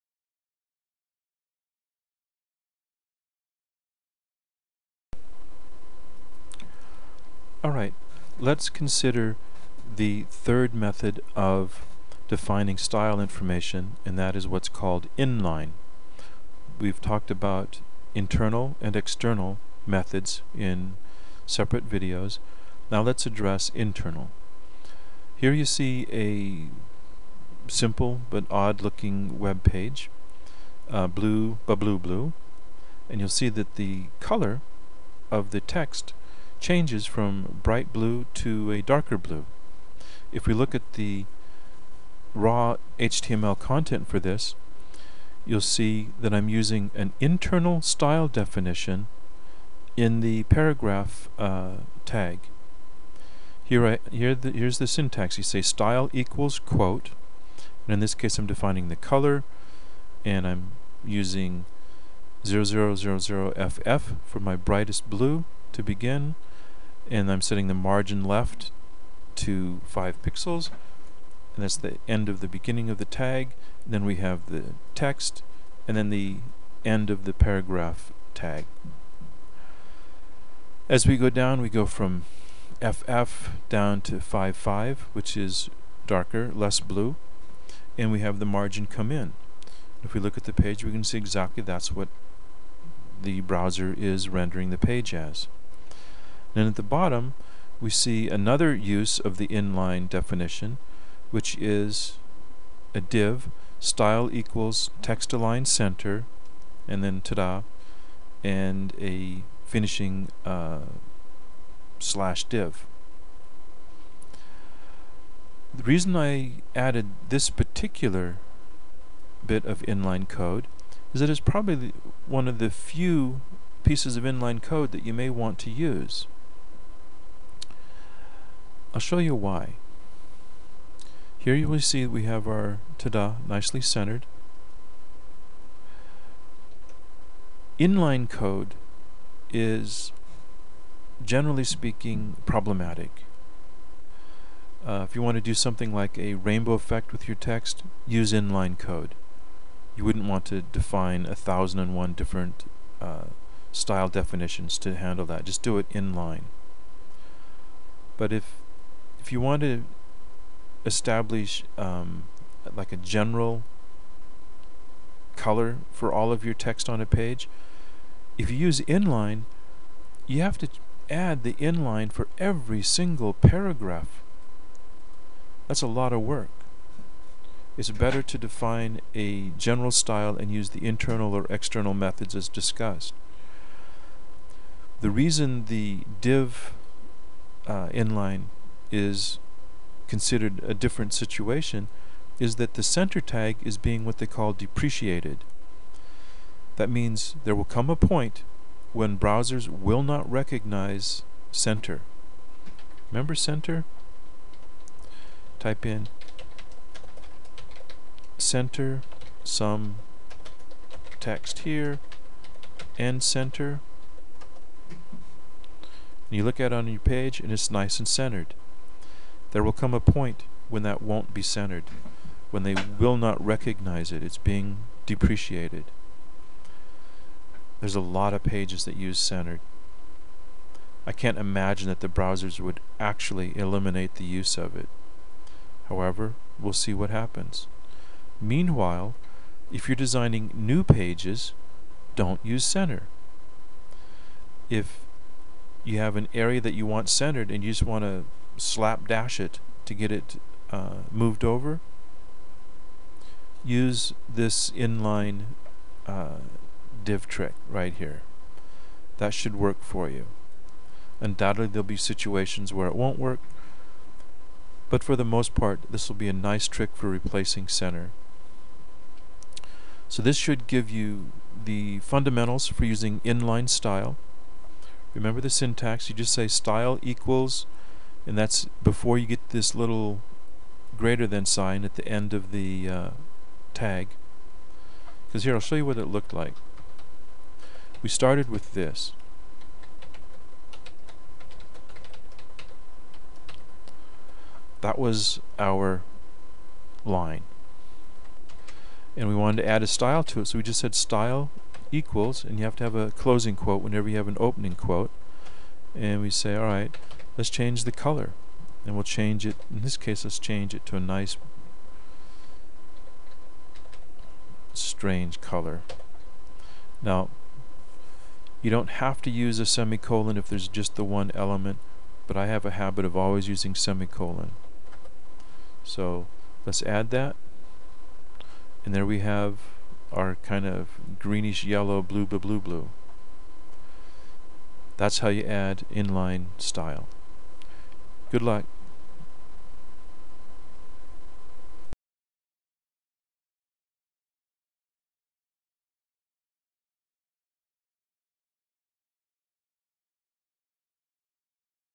CSS -- Using Inline Style Definitions
This tutorial looks at the general method for using inline style definitions. These would be styles defined right inside paragraph tags or other such fundamental tags.